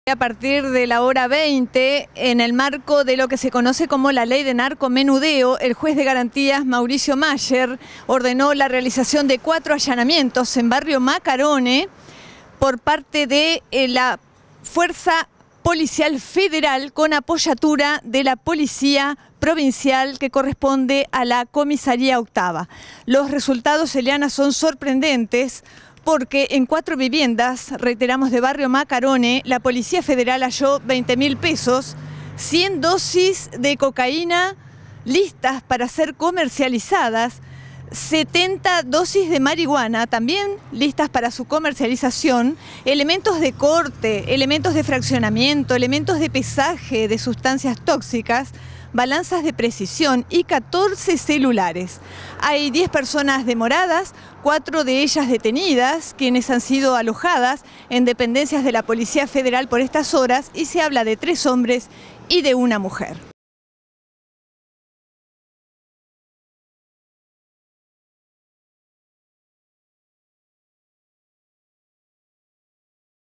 ALLANAMIENTOS BARRIO MACCARONE
INFORME ALLANAMIENTO: LA POLICÍA FEDERAL SECUESTRÓ 100 DOSIS DE COCAÍNA Y 70 DE MARIHUANA EN BARRIO MACCARONE. HUBO CUATRO PERSONAS DETENIDAS.